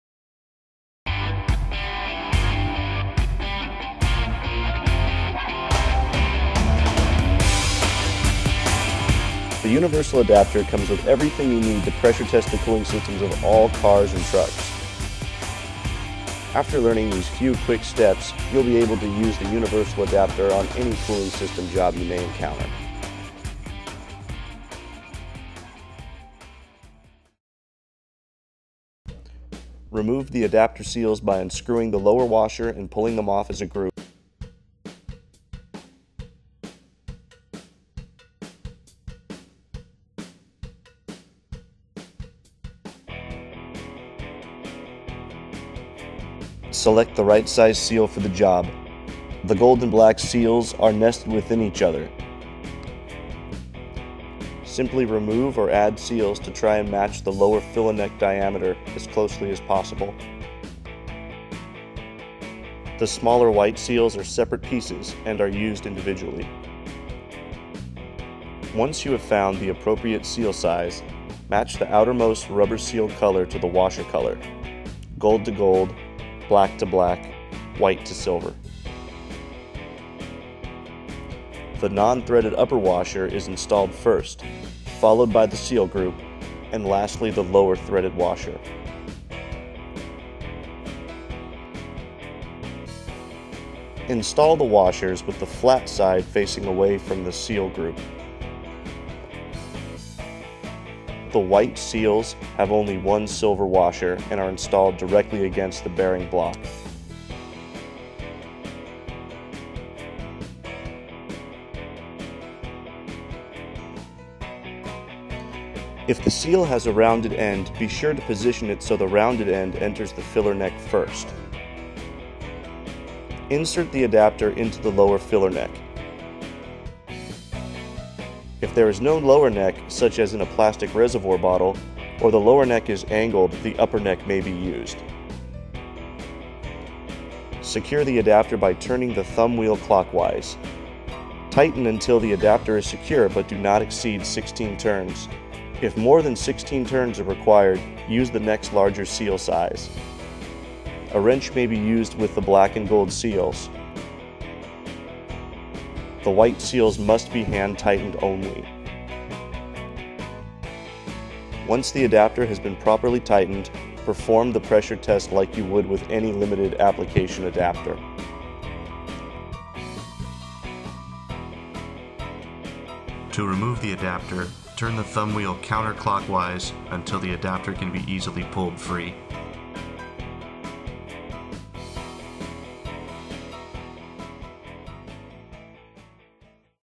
Universal Radiator/Cooling System Adapter Video
Video shows the New Universal Radiator Adapter in action. This adapter can be used on 100% of foreign and domestic cars and light truck Radiators/Cooling Systems; as well as the plastic reservoirs, regardless of inlet size or style.